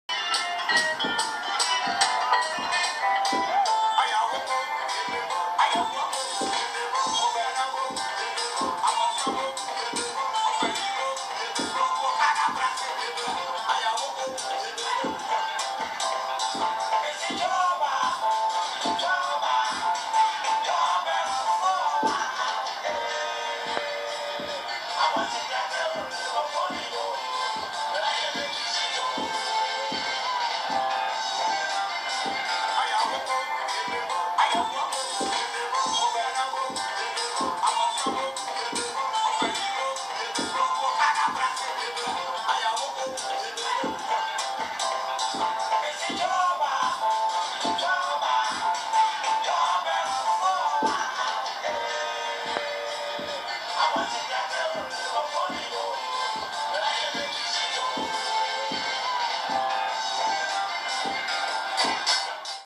Bob okala collapsed after a stage performance
The ace comedian was pronounced dead, this dawn at the Eastern Regional Hospital is reported to have collapsed after a stage performance organized by alcoholic beverages producer – Joy Herbal Industries at the Koforidua Jackson’s Park.

He was pronounced dead shortly after he was rushed to the Koforidua Government Hospital.